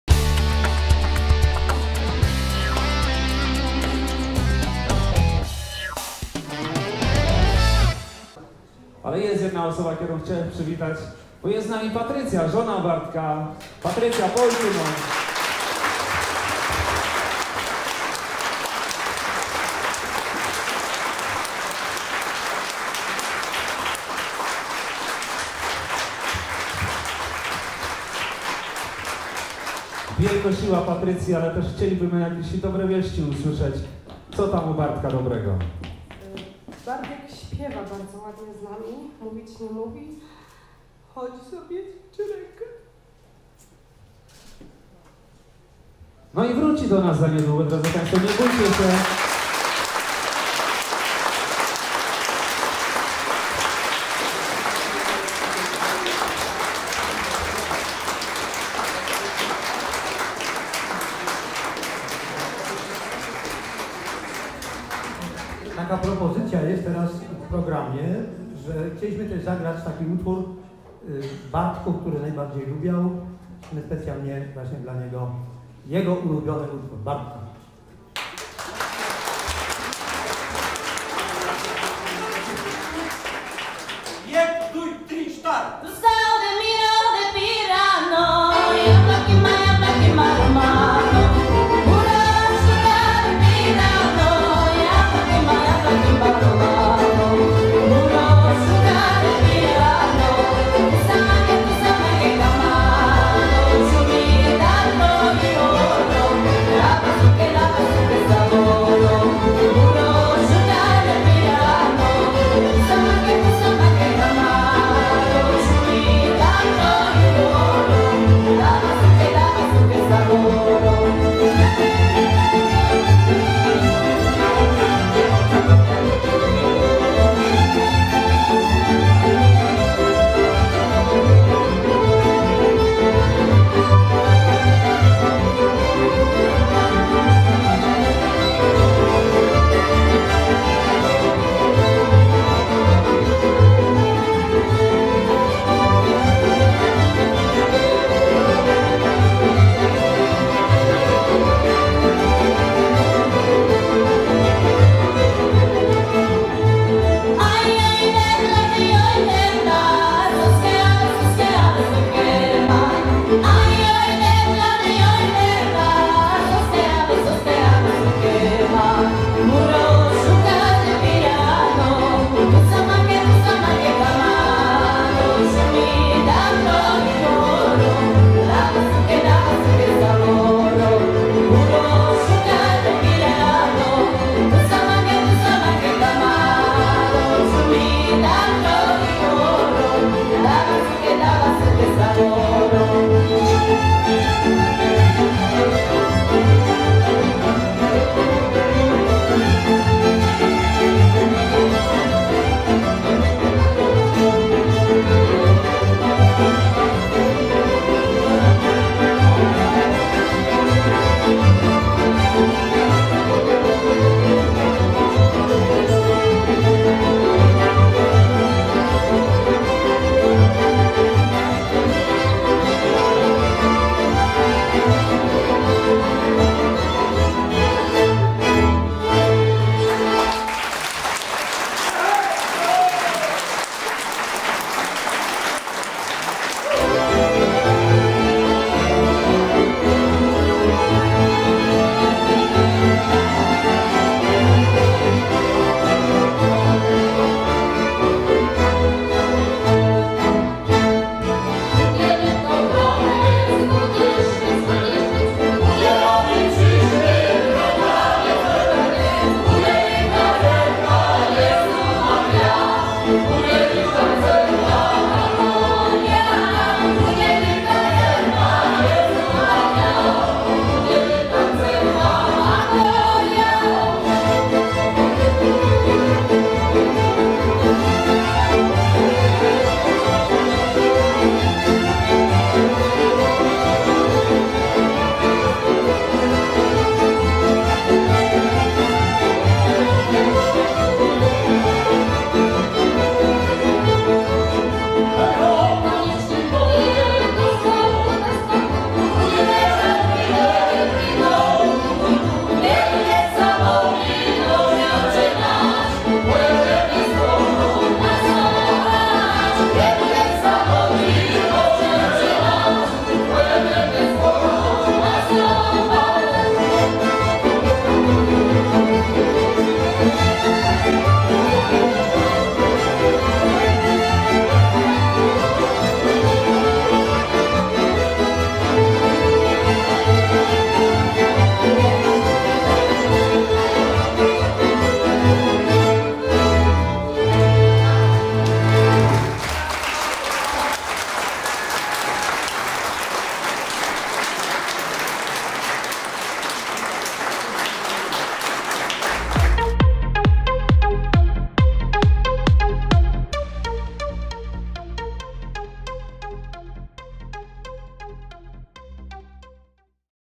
[FLESZ] SIŁA MUZYKI-Przyjaciele dla Bartka Rybki
W niedzielę (20.02) w Restauracji Dwór Kukuczka odbył się charytatywny koncert SIŁA MUZYKI-Przyjaciele dla
Bartka Rybki w trakcie, którego wystąpiły kapele: Kapela Rajwach, Zespół Koniaków i Symfonia Beskidzka.